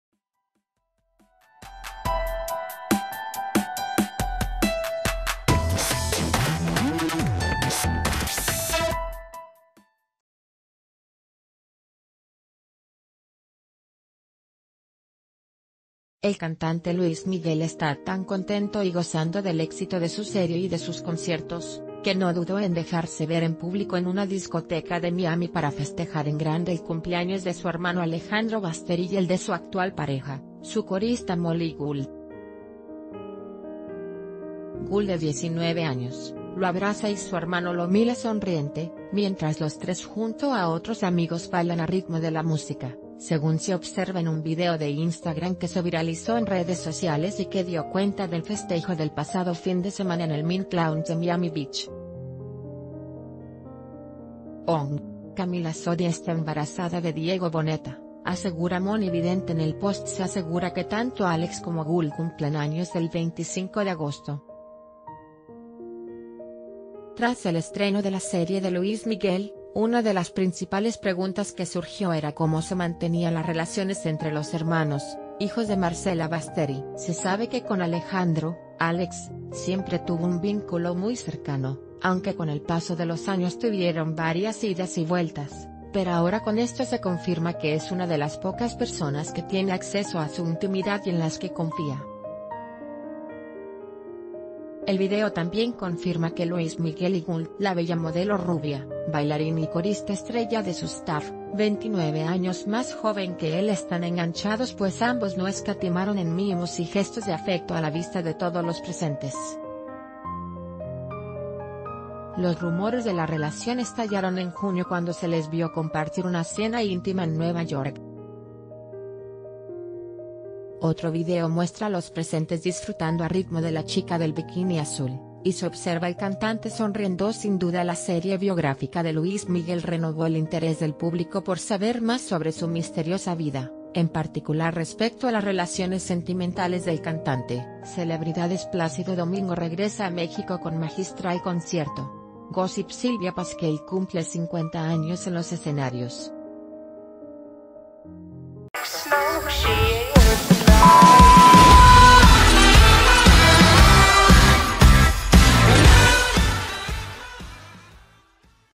Luis Miguel celebró cumpleaños de su hermano Alex en Miami
El cantante Luis Miguel está tan contento y gozando del éxito de su serie y de sus conciertos, que no dudó en dejarse ver en público en una discoteca de Miami para festejar en grande el cumpleaños de su hermano Alejandro Basteri y el de su actual pareja, su corista Mollie Gould. Gould de 19 años, lo...